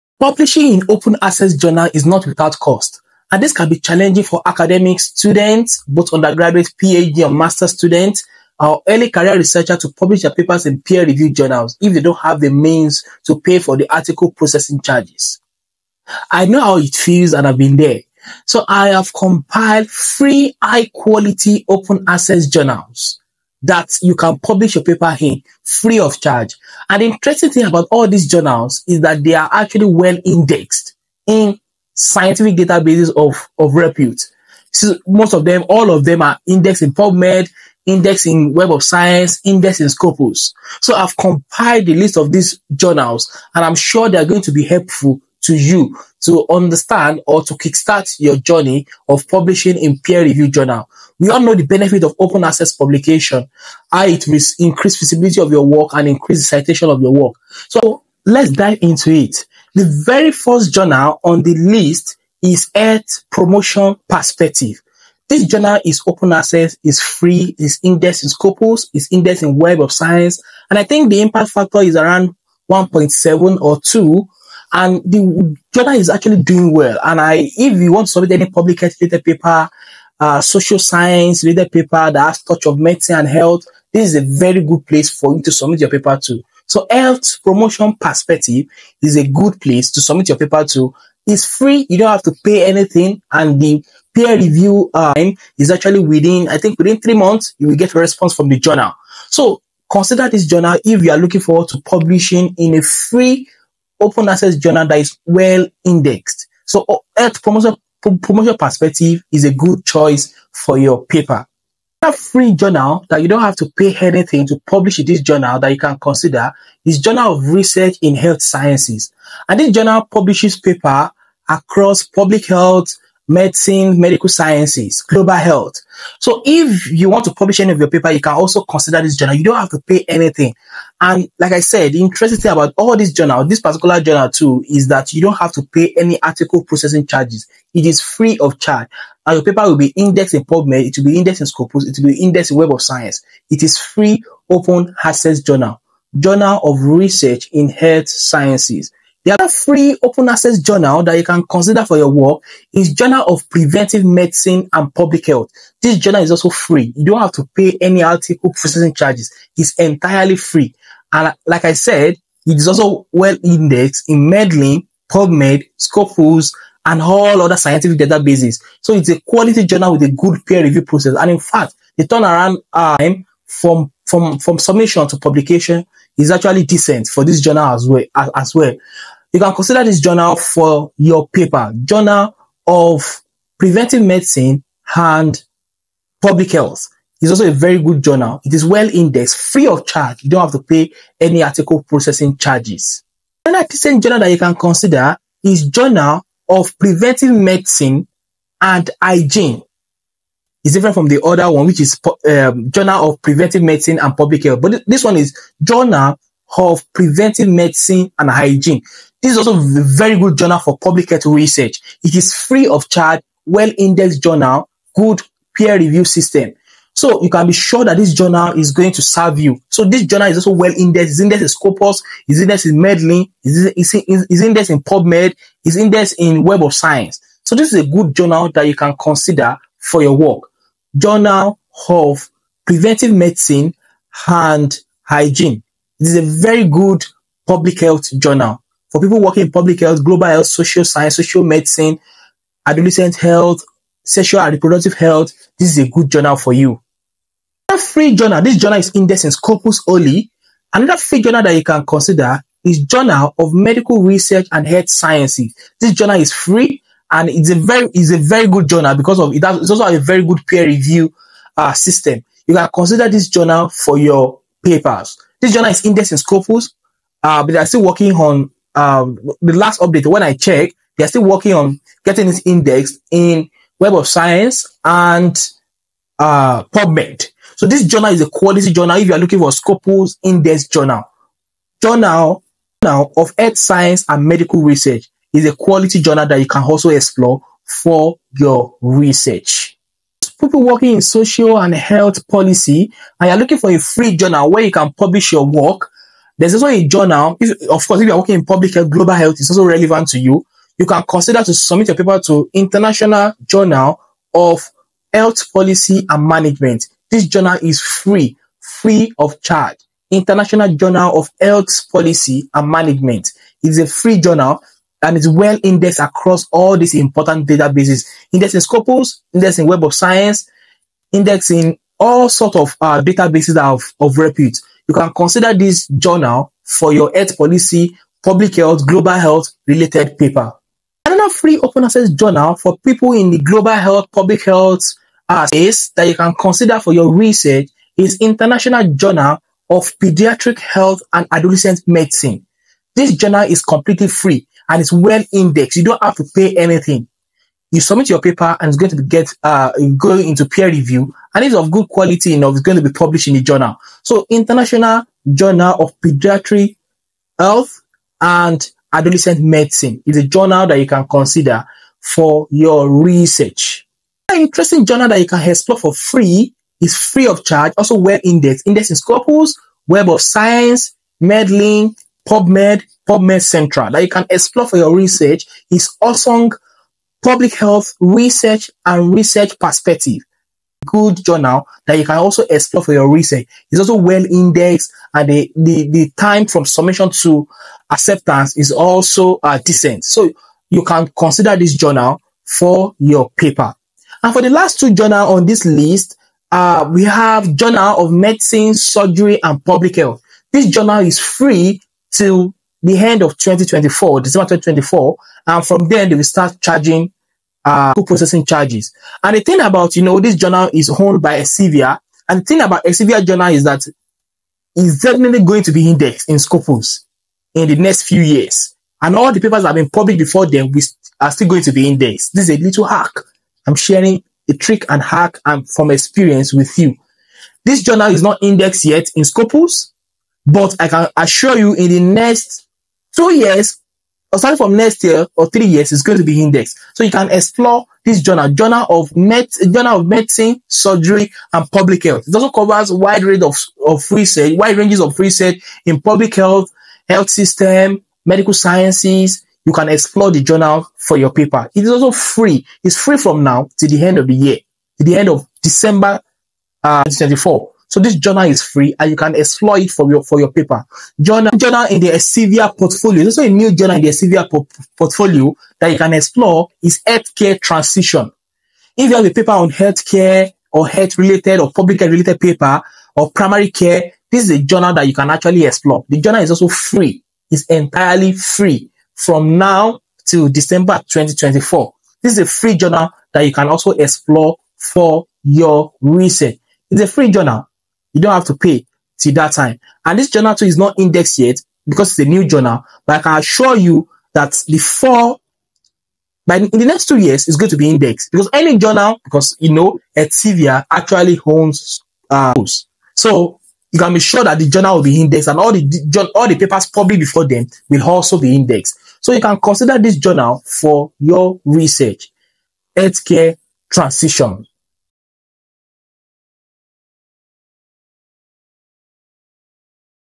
Free Quality Indexed Health/Social/Medical Sciences Journals in 2024
Web of Science, Scopus and PubMed Indexed Free (No Article Processing Charges) Journals in 2024.